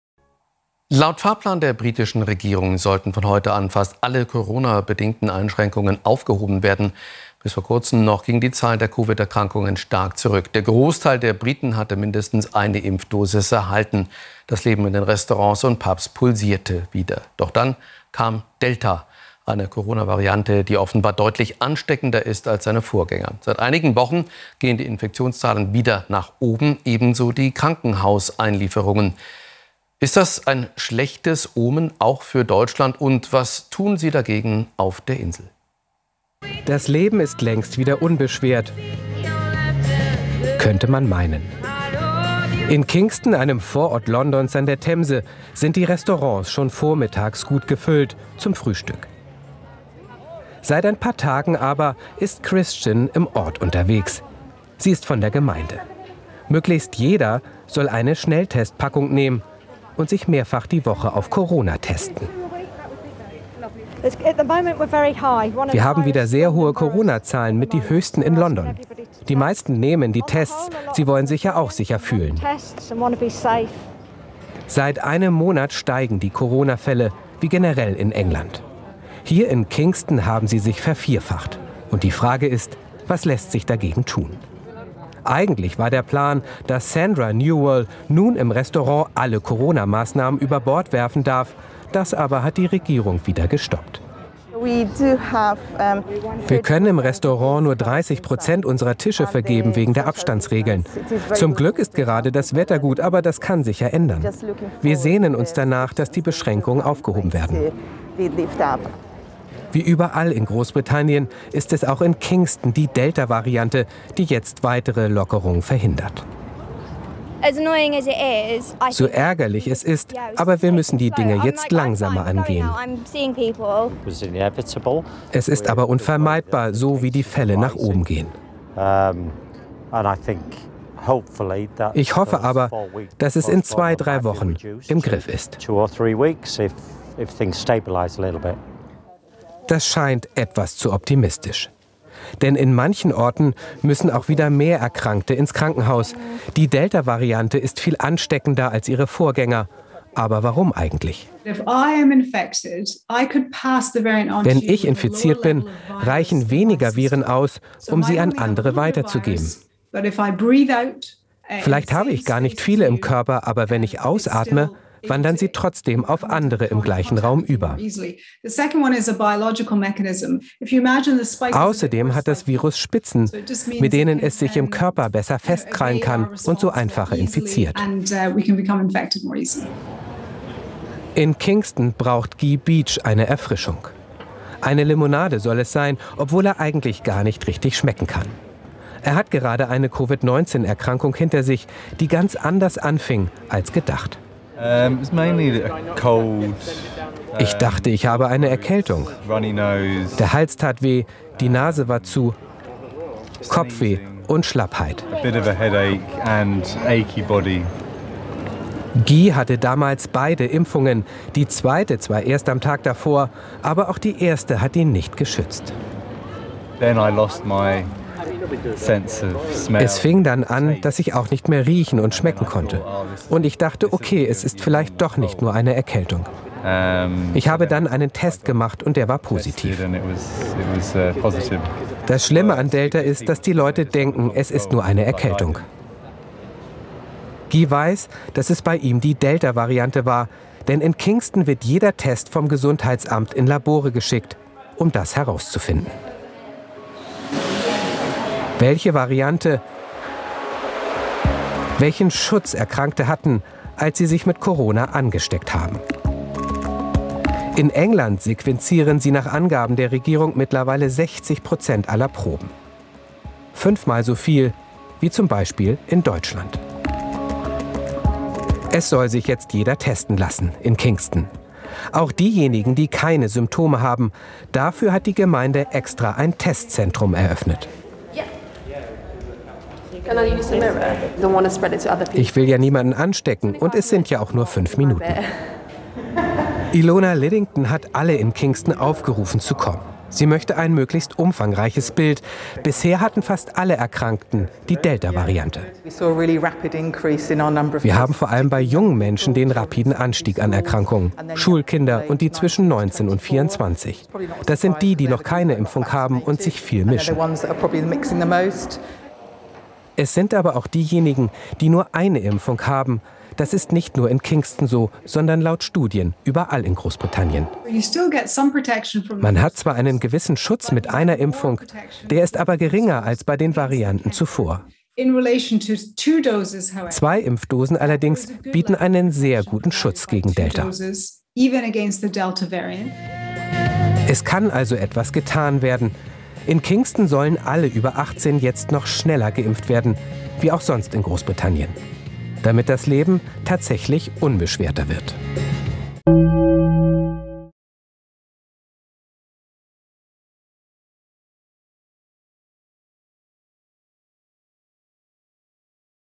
Corona in Großbritannien: Delta-Mutation breitet sich aus
Eigentlich wollte die britische Regierung von Beginn der Woche an fast alle Einschränkungen zur Eindämmung der Corona-Pandemie aufheben. Dann aber verschob Premierminister Johnson den sogenannten „Freedom Day“ um vier Wochen nach hinten auf den 19. Juli. Der Grund: Delta, eine Corona-Variante, die offenbar deutlich ansteckender ist als seine Vorgänger. Seit einigen Wochen gehen die Infektionszahlen wieder nach oben, ebenso die Krankenhaus-Einlieferungen. In Großbritannien ist Delta bereits die dominierende Variante.